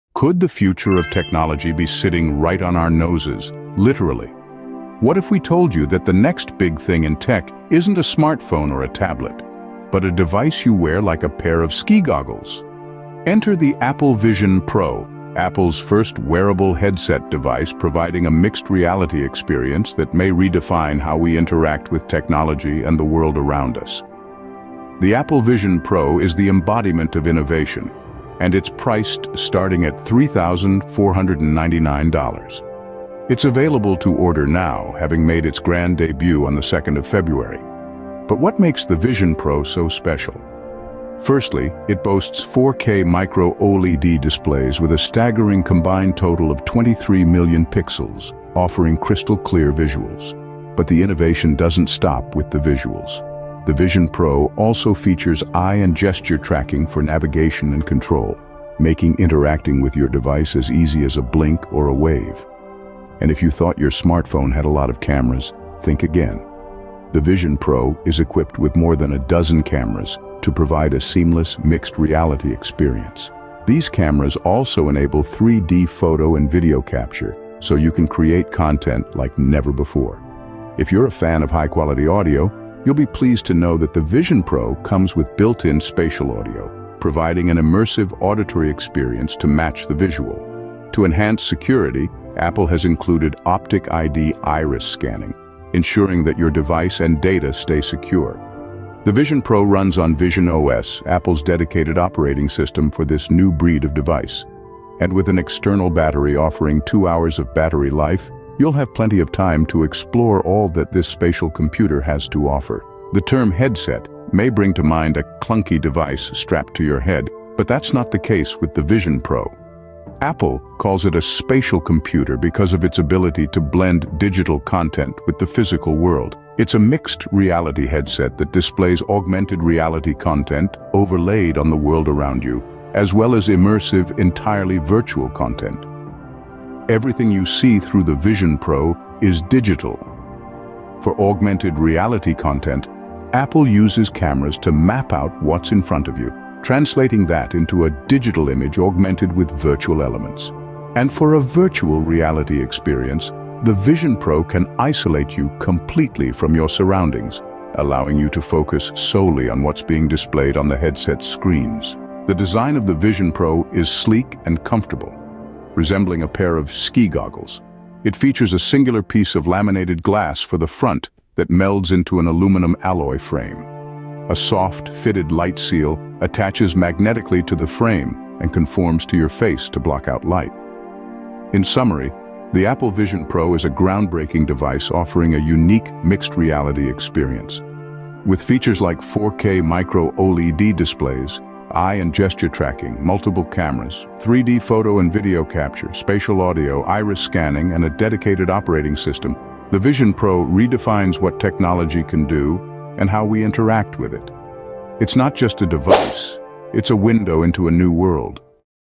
Apple Vision Pro: The Future of Mixed Reality 🎧📳
Welcome to the future of Mixed Reality with Apple Vision Pro! 🌐 Explore the cutting-edge technology, seamless integration, and immersive experiences that redefine the way we perceive reality. Dive into the world of augmented and virtual reality like never before. Uncover the innovative features, sleek design, and limitless possibilities that make Apple Vision Pro a game-changer in the realm of Mixed Reality.